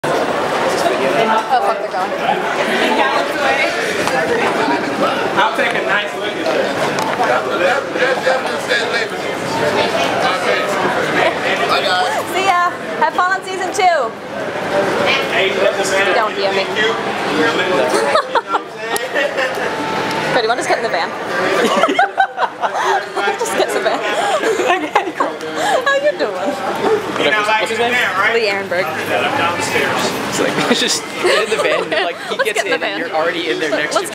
Lee Arenburg leaving Dragoncon 2012
He was a really nice guy, it was a pleasure to meet him--I kept seeing him all weekend!